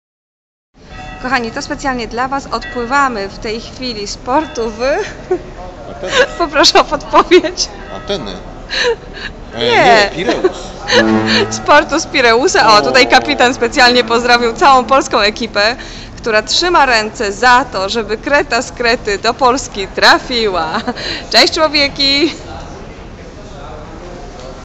Wypływamy z portu w Pireusie do Heraklionu.
Wybaczcie zmęczenie... kciuki- nie ręce! :) Psiodróż nadziei- Kreta z Krety